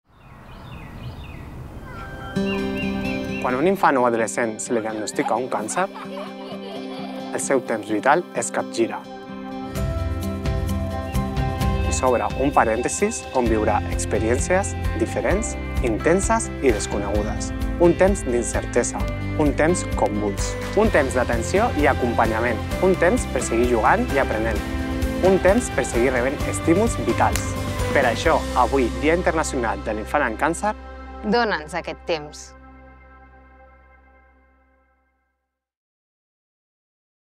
AFANOC: Dia Internacional de l'Infant amb Càncer "Dona'nsTemps"
Quan a un infant se li diagnostica un càncer, la gestió del seu temps serà determinant a l’hora de fer front al procés que viurà. Li caldrà atenció i acompanyament que l’ajudin en el nou dia a dia.

Des de l’AFANOC enfoquem aquest acompanyament respectant els seu moment vital i procurant que la malaltia no l’impedeixi seguir jugant, aprenent, o estudiant.

Que, en la mesura del possible, pugui viure les experiències i els estímuls imprescindibles per la seva formació i pel seu creixement.

Des dels hospitals, des de casa i a la tornada a l’escola.
Avui, Dia Internacional de l’Infant amb Càncer, volem posar l’accent  en aquest acompanyament, des de tots els àmbits del seu entorn, que junts puguem donar-li un temps de qualitat.

Ajuda’ns a donar-li aquest temps!

➡ AJUDA'NS a seguir: BIZUM amb el codi 00542 o al web

Cuando a un niño se le diagnostica un cáncer, la gestión de su tiempo será determinante en la hora de hacer frente al proceso que vivirá. Le hará falta atención y acompañamiento que lo ayuden en el nuevo día a día.

Desde AFANOC enfocamos este acompañamiento respetando los suyo momento vital y procurando que la enfermedad no lo impida seguir jugando, aprendiendo, o estudiante.

Que, en lo posible, pueda vivir las experiencias y los estímulos imprescindibles por su formación y por su crecimiento.

Desde los hospitales, desde casa y a la vuelta en la escuela.
Hoy, Día Internacional del Niño con Cáncer, queremos hacer hincapié en este acompañamiento, desde todos los ámbitos de su entorno, que juntos podamos darle un tiempo de calidad.

Ayúdanos a darle este tiempo!

➡ AYÚDANOS a seguir: BIZUM con el código 00542 o en la web